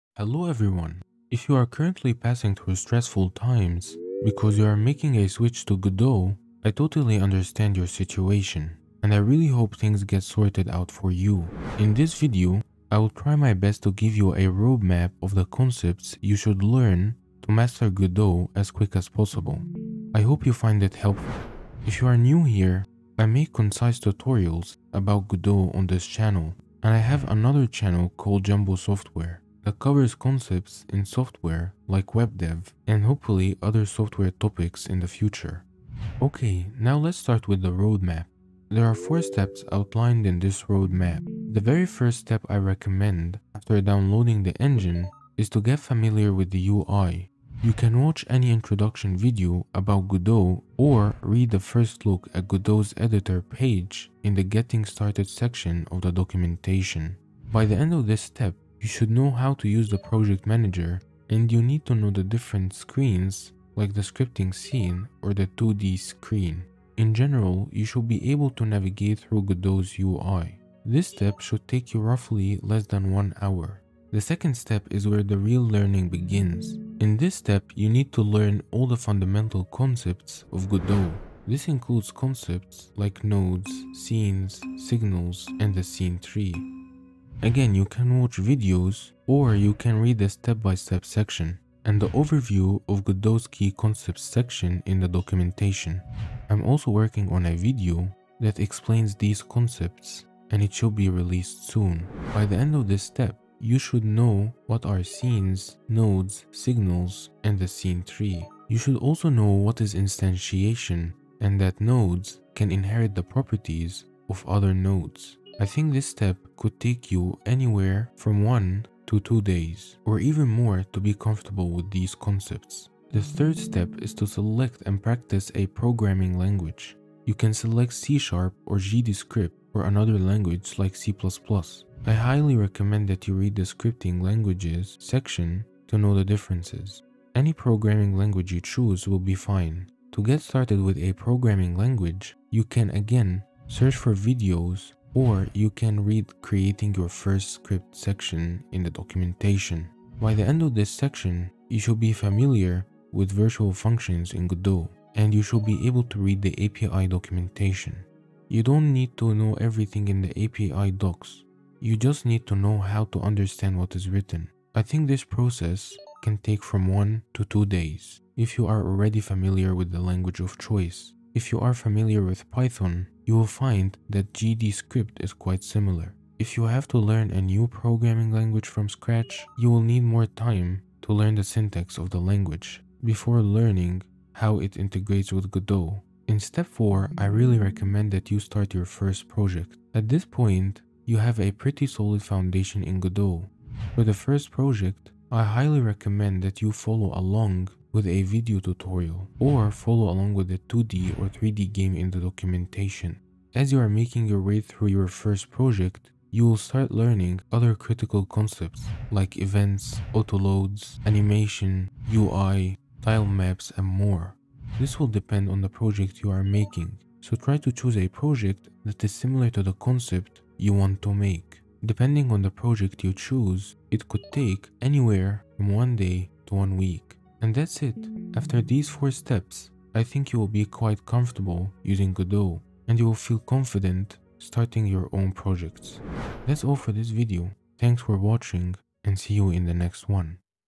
Roadmap to learn Godot 4 as quick as possible
In this video, I highlight what I think is the fastest way to learn and get comfortable using Godot, allowing you to work on your projects as soon as possible.